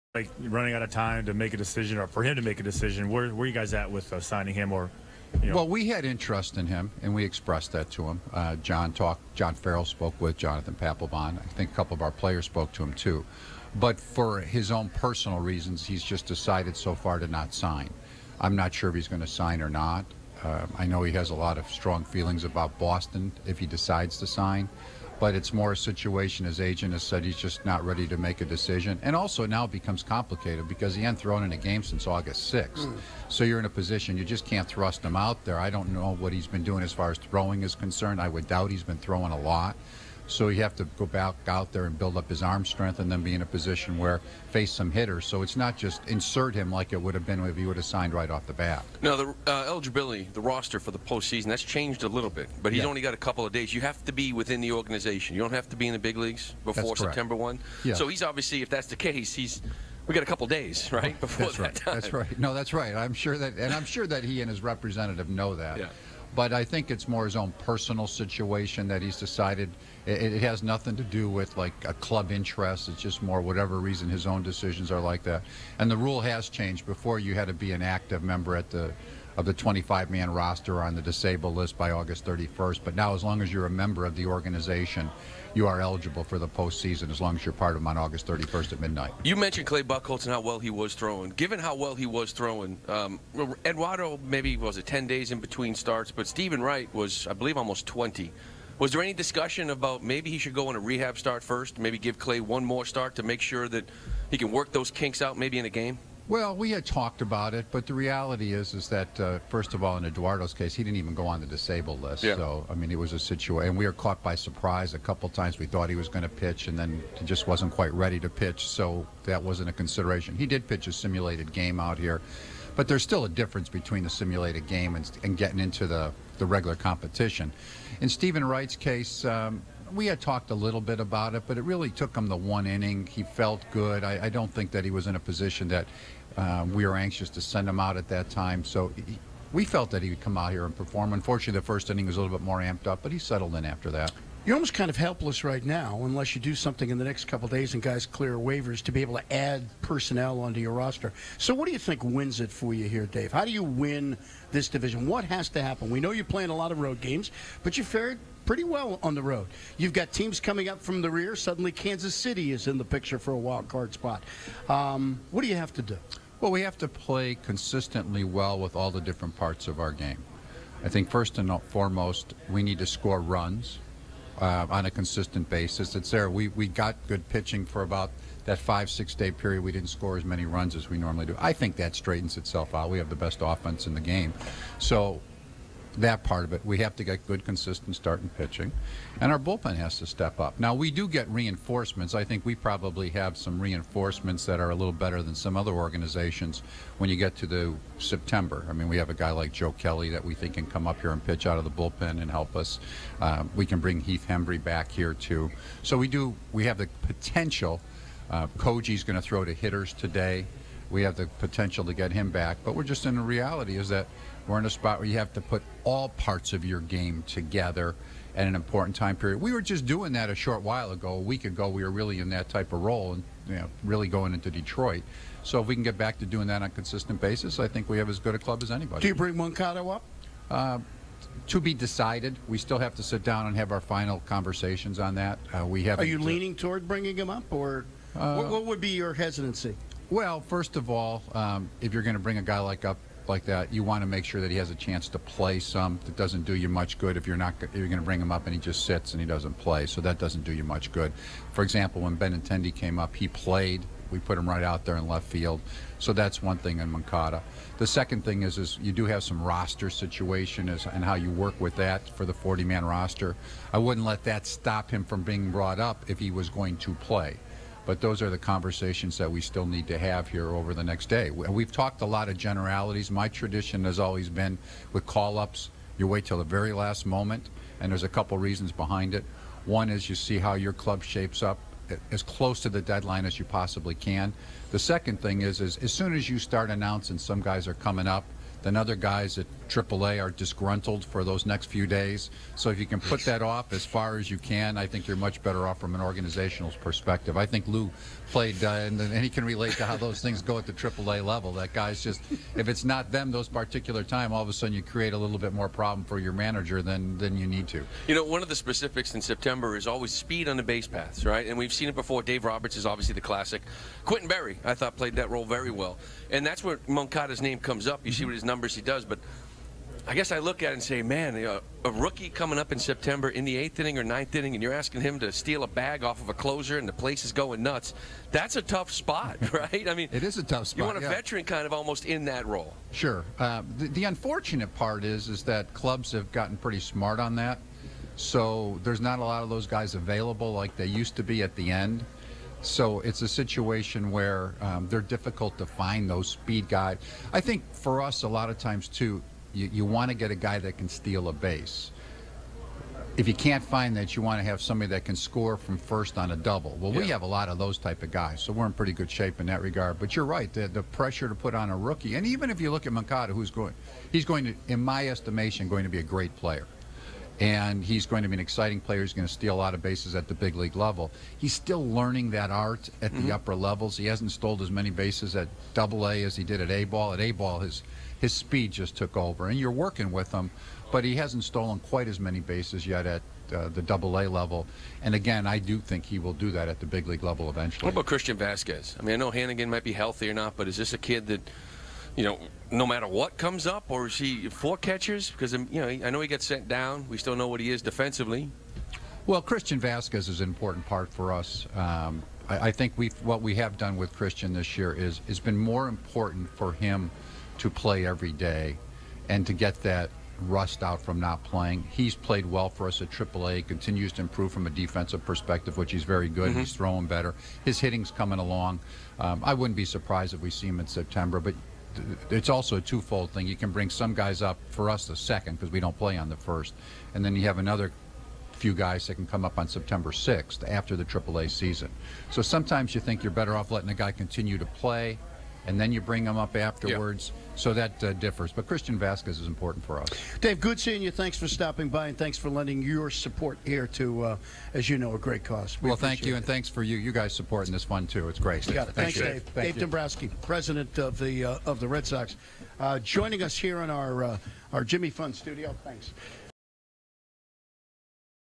Jimmy Fund: Dave Dombrowski
Boston Red Sox general manager Dave Dombrowski joins the WEEI NESN Jimmy Fund telethon.